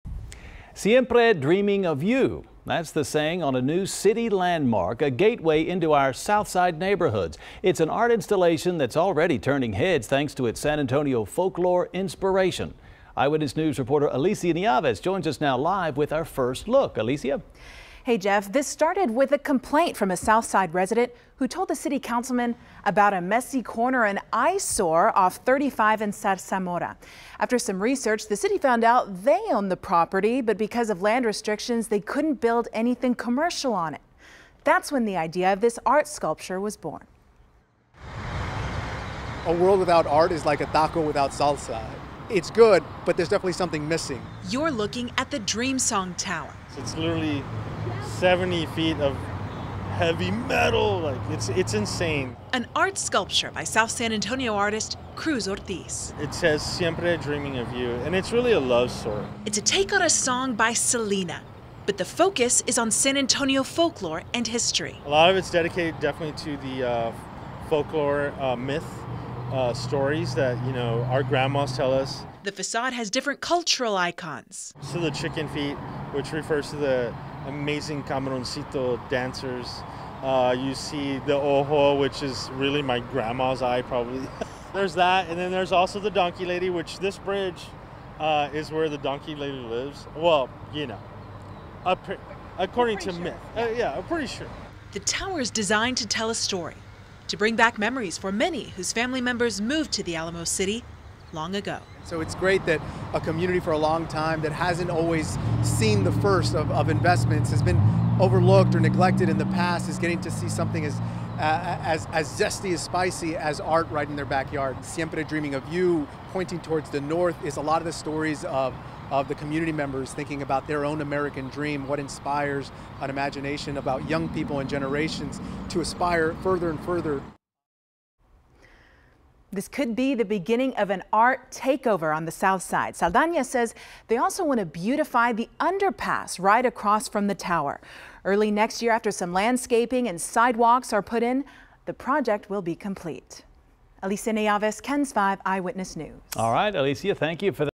South-side 'Dream Song Tower' gives nod to San Antonio legends and folklore
"Siempre Dreaming of You" is the saying on a new city landmark, a gateway into south-side neighborhoods.